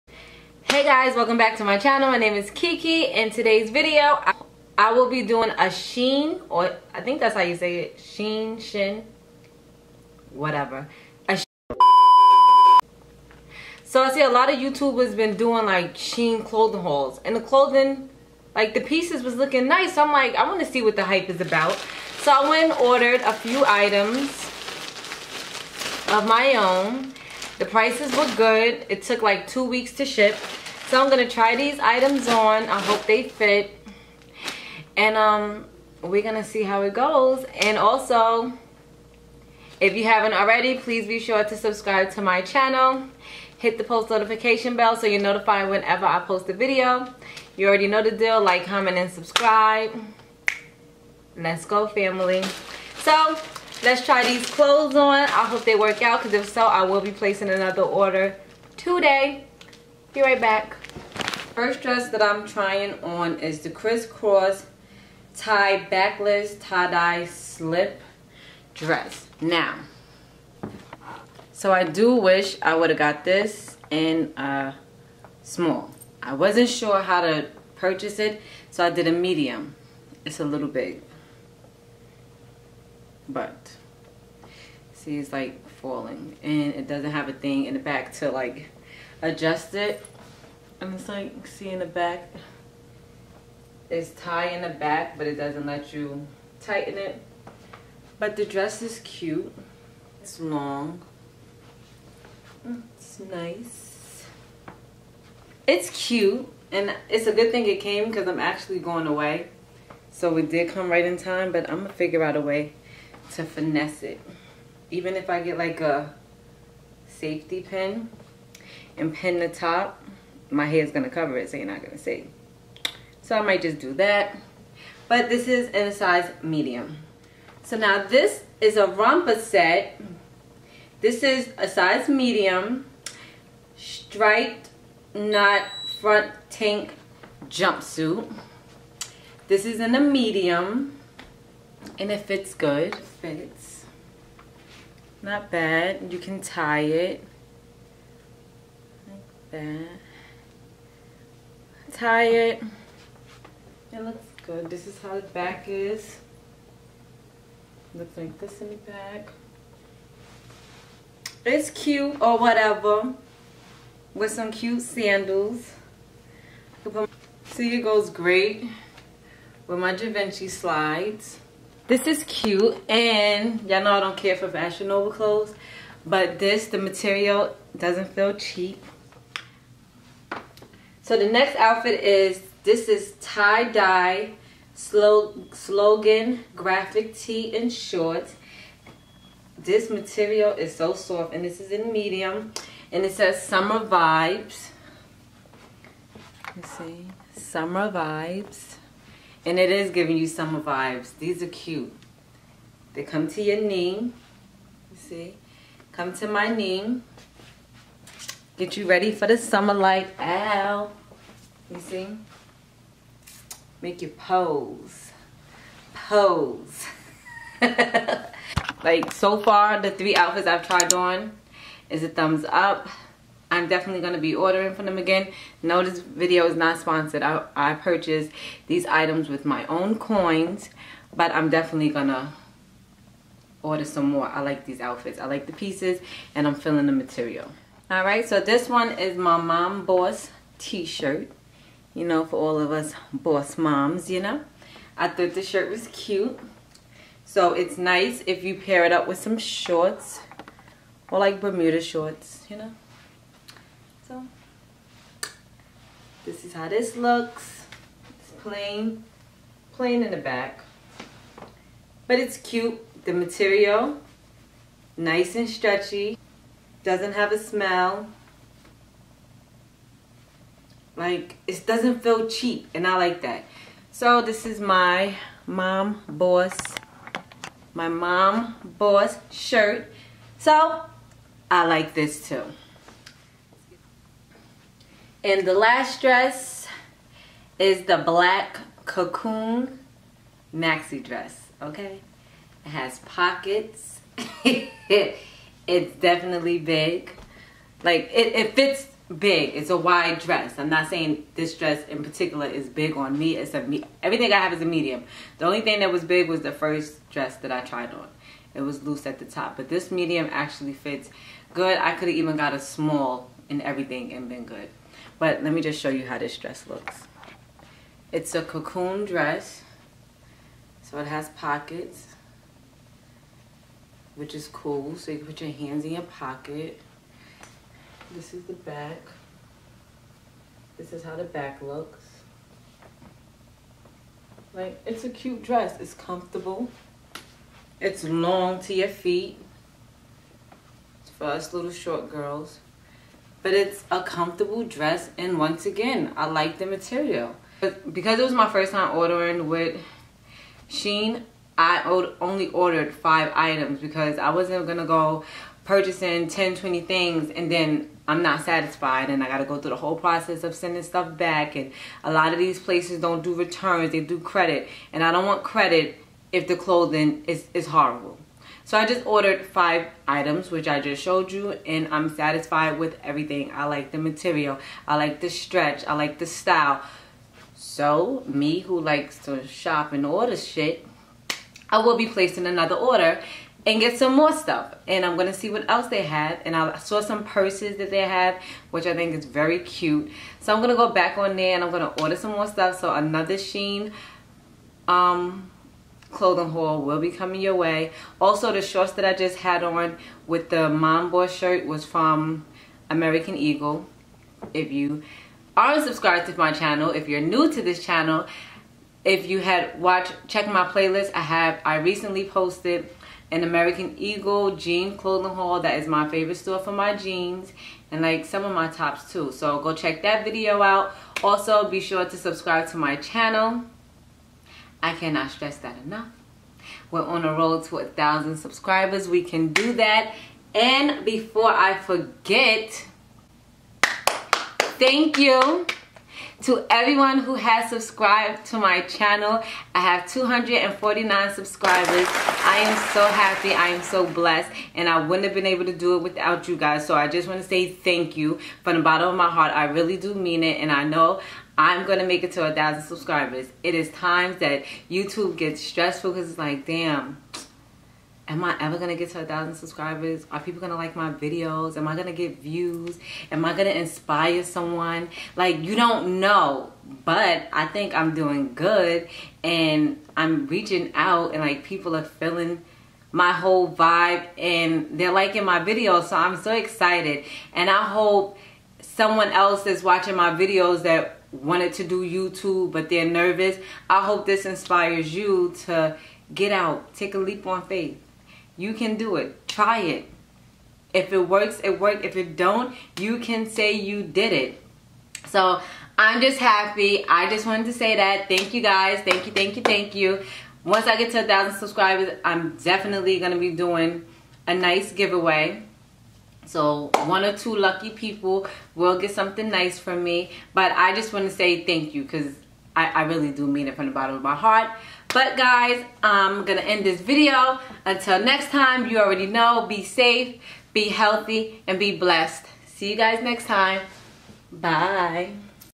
Shein Try On Haul | Honest Opinion
Heyyyy My Loves! Today I will be doing a Shein clothing haul. This is my first time purchasing items from this company, so lets see what the hype is about. Is the clothes good quality or NO??? Watch the video to find out.

Items Purchased:
CrissCross Tie BackLess Tie Dye Slip Dress (M) $16.01
Leopard & Letter Graphic Tee (M) $8.47
Striped Knot Front Tank Jumpsuit (M) $13.18
Tie Dye Slogan Graphic Tee & Shorts (M) $13.18
Cocoon V-Neck Cami Maxi Dress (M) $14.12

🌟 To Book an appt for lashes click the link below 🌟

" In Order To Be Irreplaceable, One Must Always Be Different." CoCo Chanel

TikTok lavish looks_bykiki